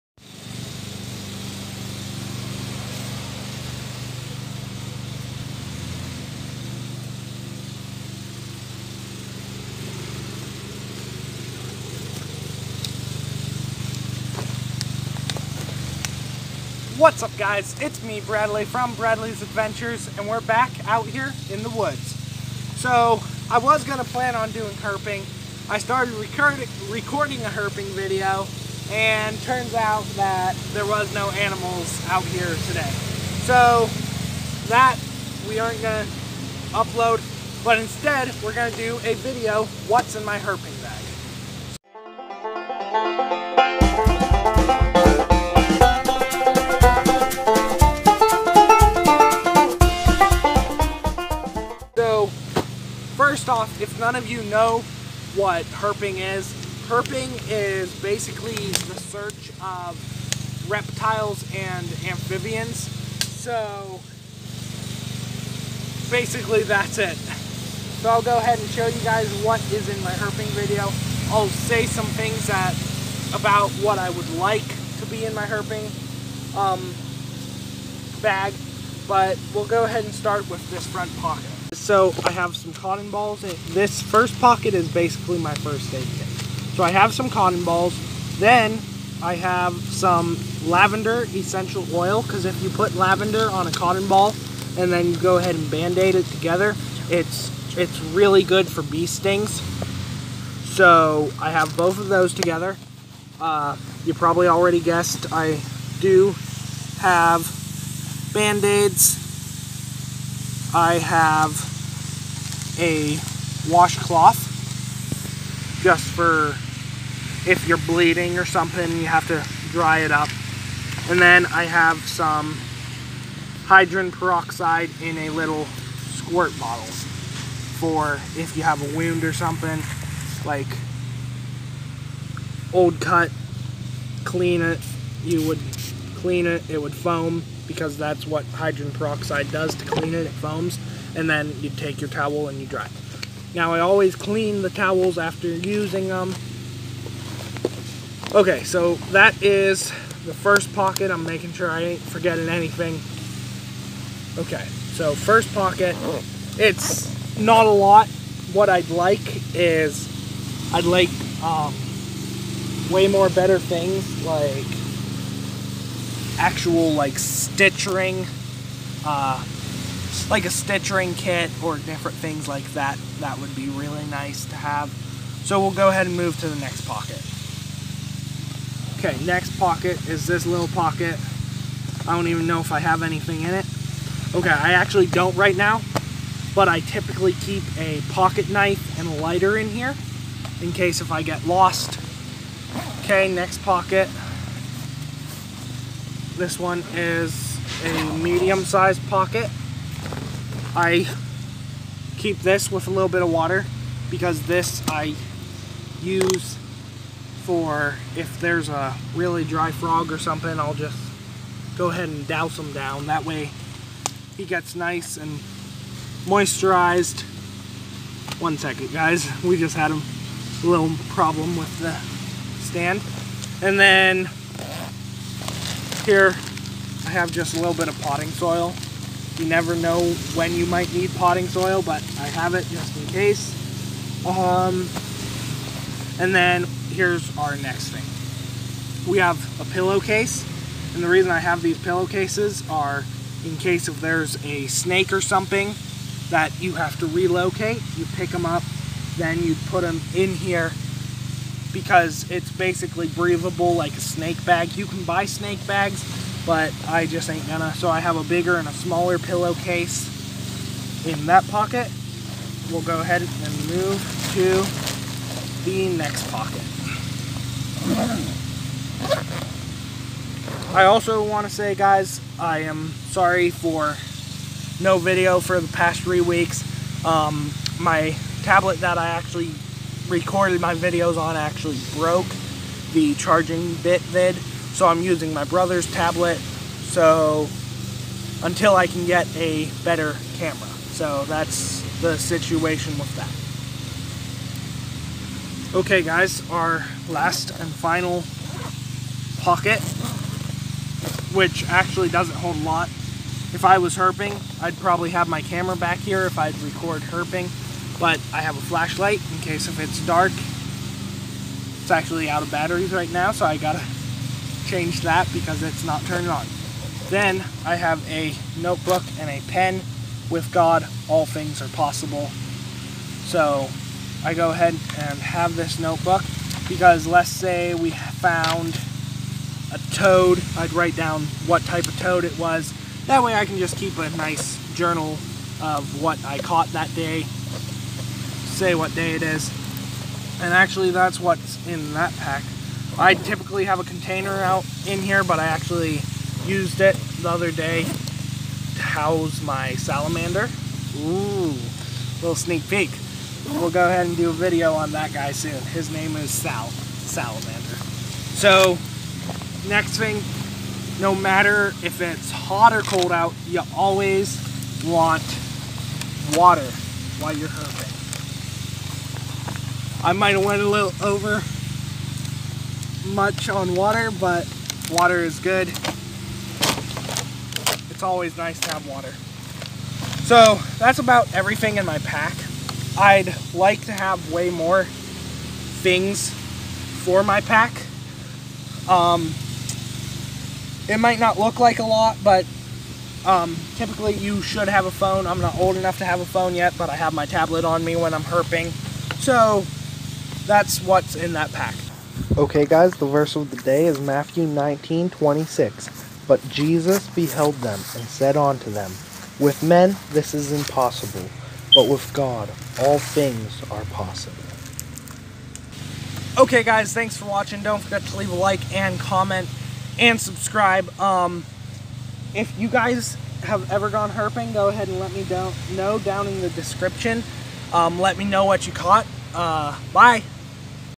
What's in my herping bag?!?!
In this video we look what  is in my herping bag